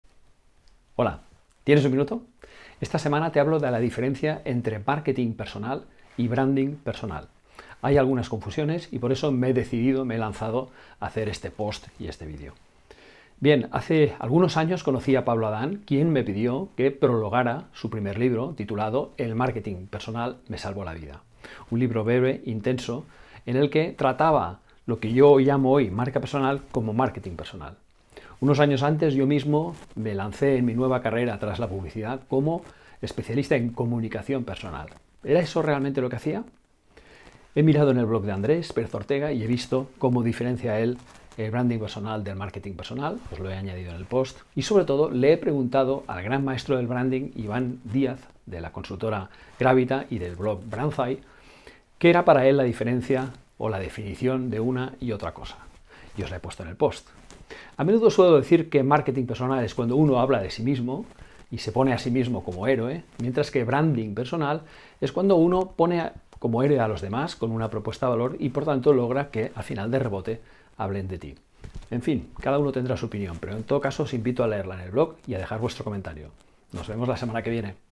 Branding Personal y Marketing Personal ¿es lo mismo?
Guillem Recolons trata la diferencia entre marketing personal y Branding personal. Hay algunas confusiones, fruto a menudo del propio desconocimiento de los significados de Branding y marketing. Para ello, se han utilizado ejemplos de Pablo Adán, de Andrés Pérez Ortega y del especialista en Branding (Branzai, Grávita) Iván Díaz.

Lo cierto es que una disciplina no es mejor que la otra. De hecho, el marketing personal es una parte del proceso del Branding personal. En el post están todas las definiciones junto a un símil sobre la idea del viaje del héroe, muy utilizada en la técnica del relato.